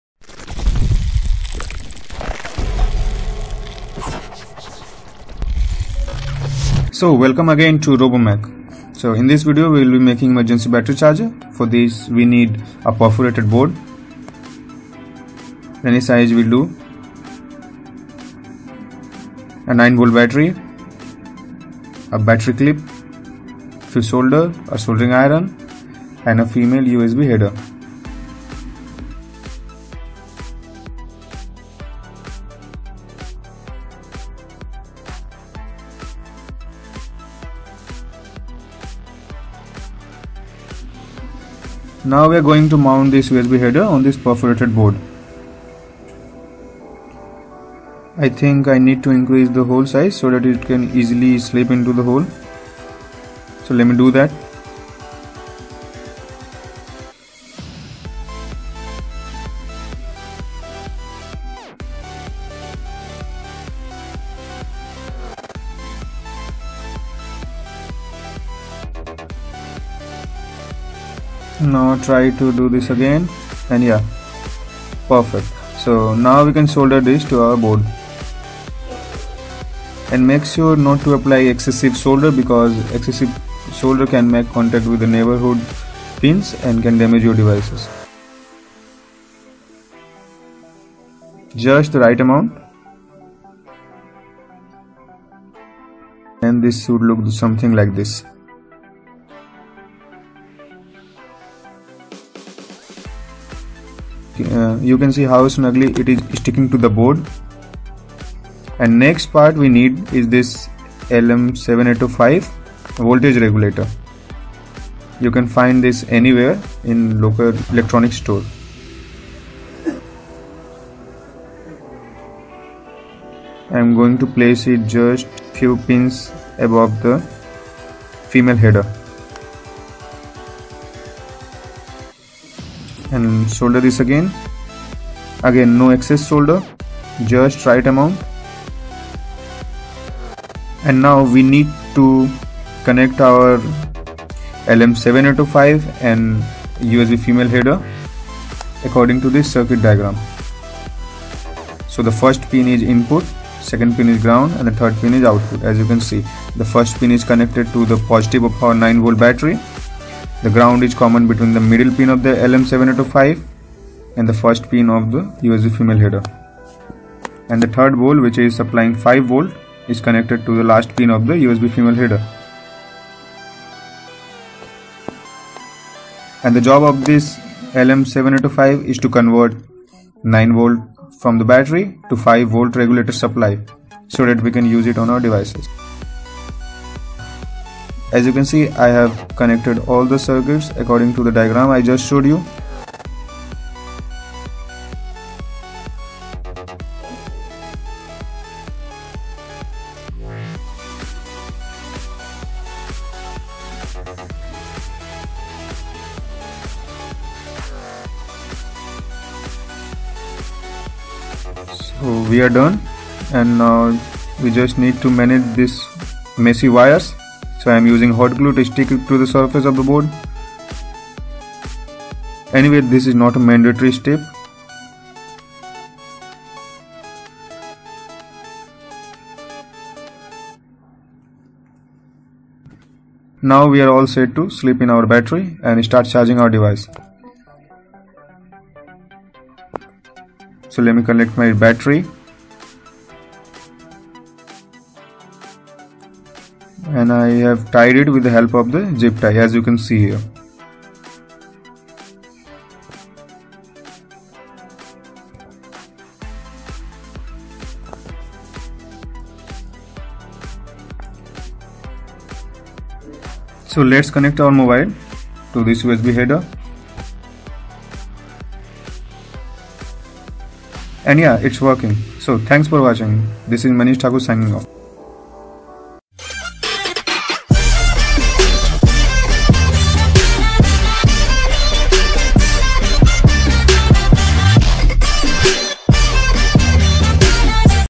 DIY Emergency Power Bank / Phone Charger - Quick Charge for under 0.4 $
in this video, i will make a phone charger for under 0.4 $

Parts required :

1) LM7805
2) 9 V battery with clips
3) Few wires
4) Soldering Iron and Soldering wires

and just follow the steps..